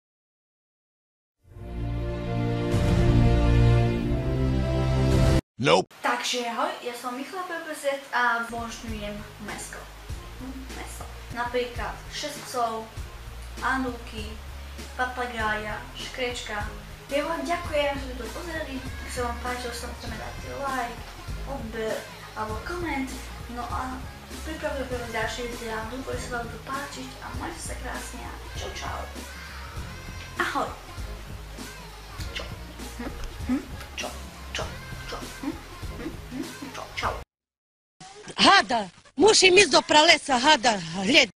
[SK/CZ]Michalppz je psov!!!
O toto si si michal vážne koledoval, kedysi som to chcel spraviť a teraz ma to napadlo ako si spravil to video Pozerám svoje staré videa :D. Celé video som zostrihal z jeho staršieho vlogu nič viac. :D